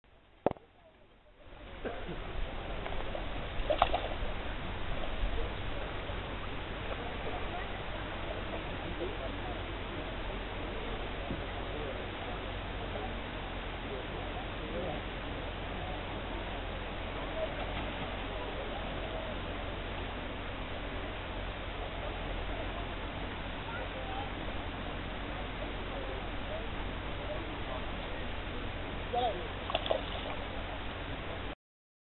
Rybár a jeho úlovok (fisherman)
Rybár a jeho úlovok na Pozlovickej priehrade dňa 5.8.2015